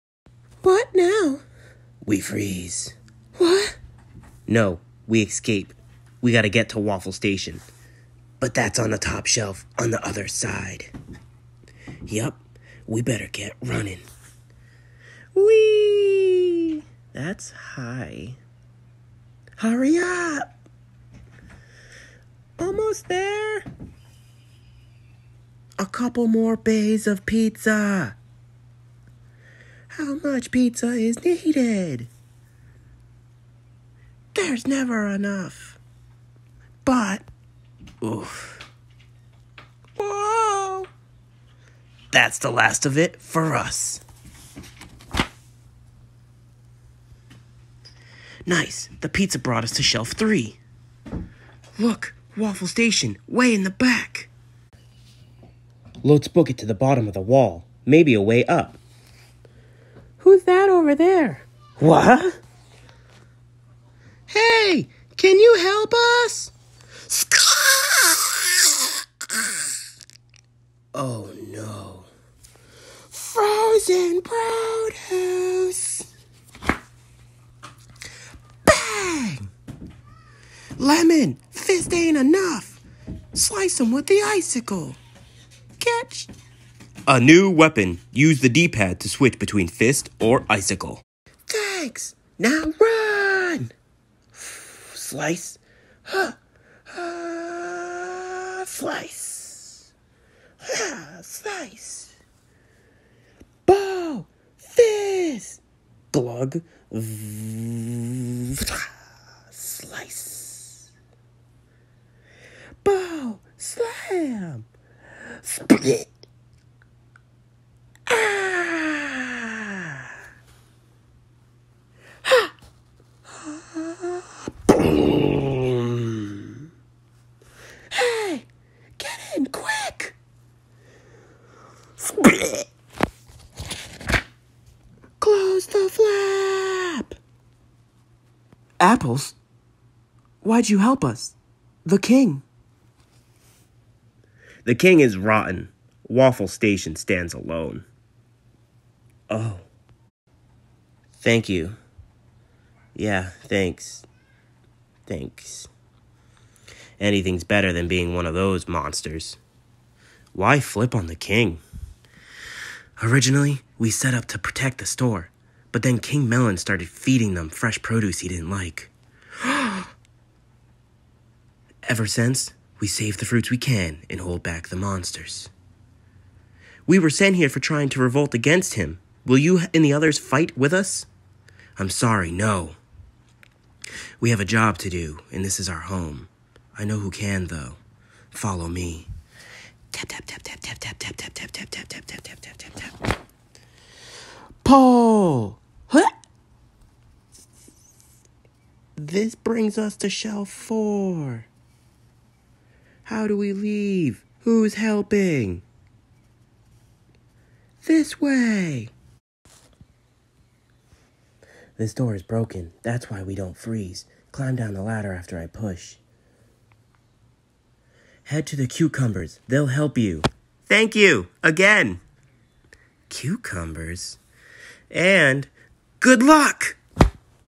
THE LAST LEMON PT6
THE LAST LEMON PART 6
Now in color!!

Lemon and the gang learn about the store's freezer.